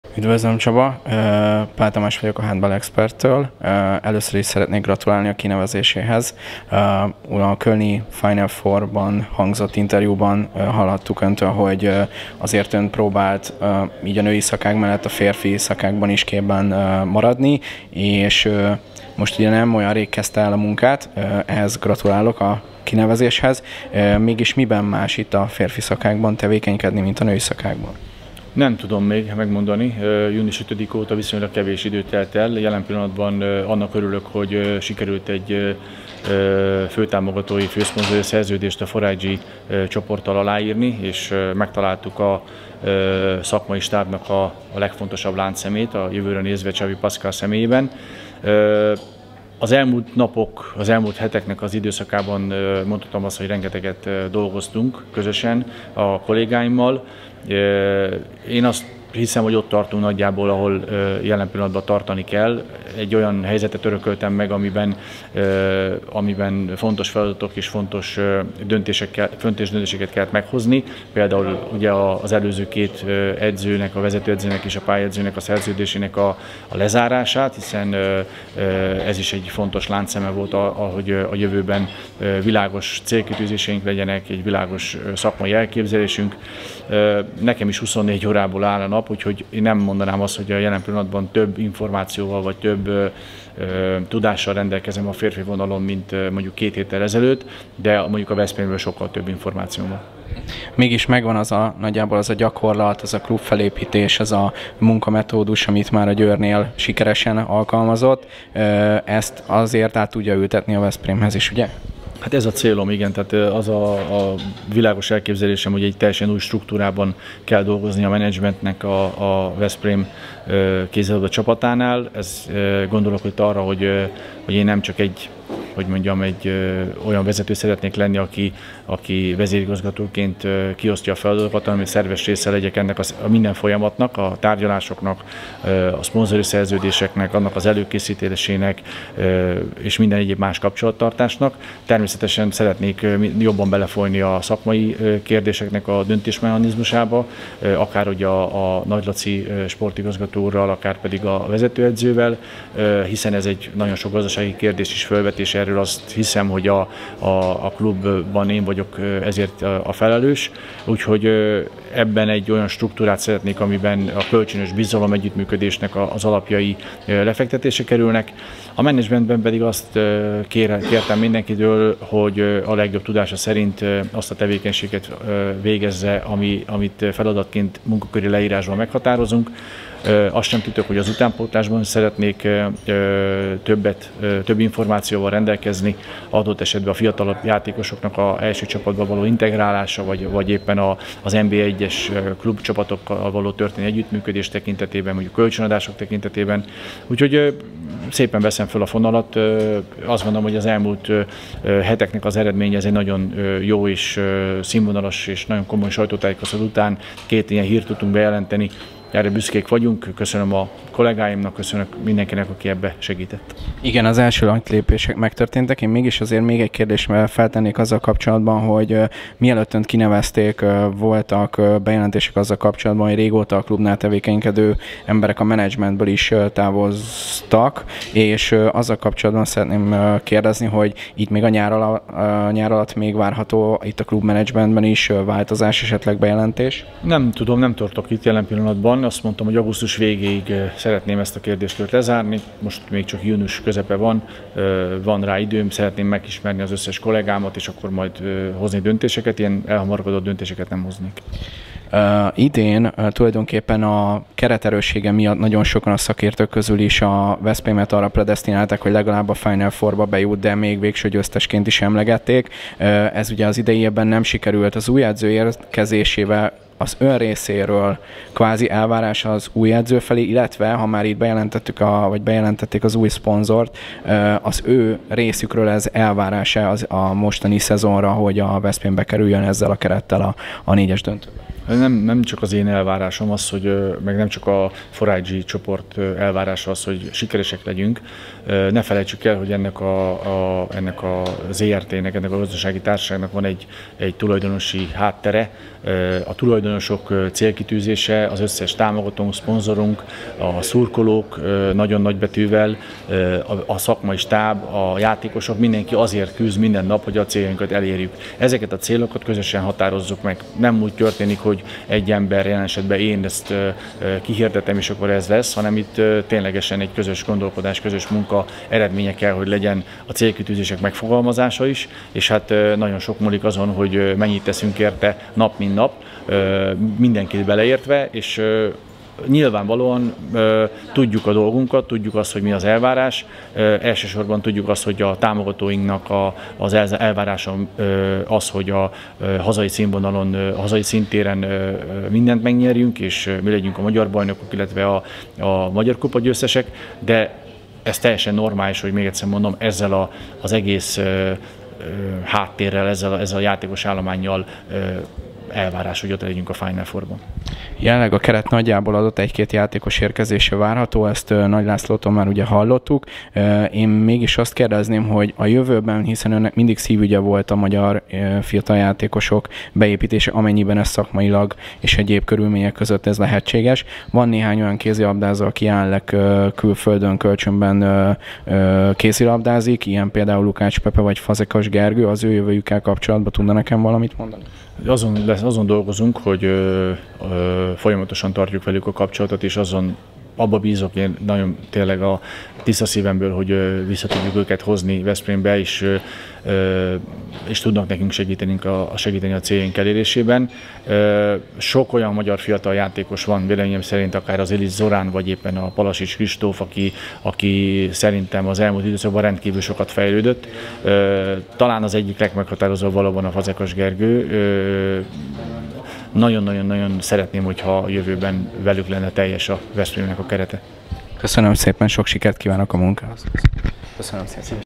Dr. Bartha Csaba: Magyar fiatalokkal lenne teljes a Veszprém kerete
Új vezér, új edző, új szponzor - új éra?
Ott jártunk a magyar rekordbajnok sajtótájékoztatóján. Kollégánk, Pál Tamás, a Veszprém Handball Team Zrt. vezérigazgatójával készített interjút.